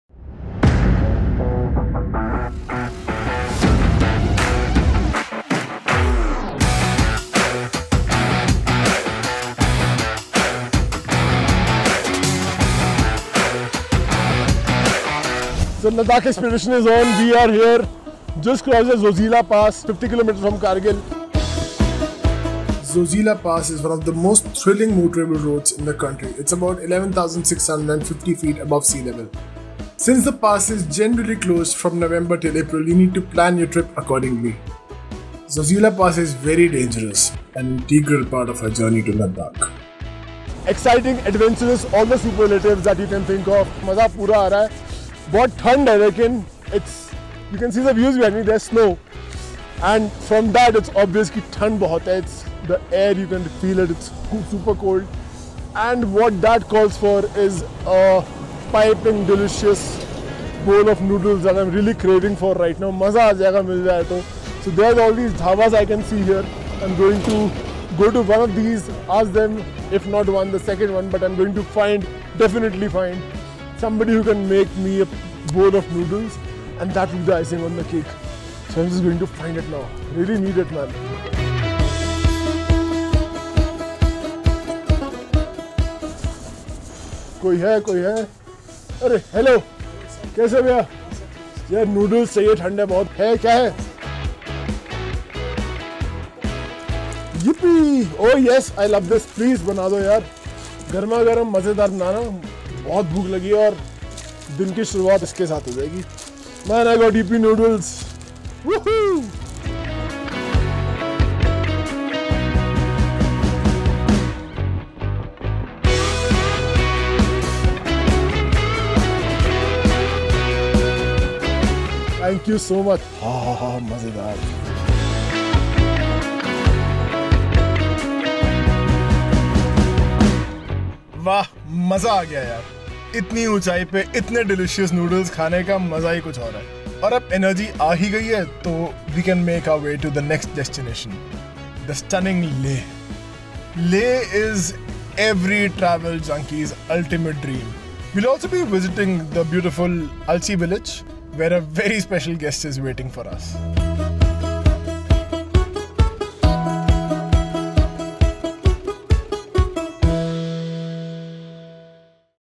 A Special Meal At Zojila Pass With Sunfeast Yippee! Noodles
11 days, 9 places, one unforgettable journey.

Our expedition to Ladakh kickstarted on a truly high note - literally! As we made our way to Zoji La Pass, one of the most dangerous mountain roads in the world, we found ourselves at an elevation of 11,500 feet.
'It was here where our Food Trails began, with a delicious bowl of Sunfeast Yippee! Noodles'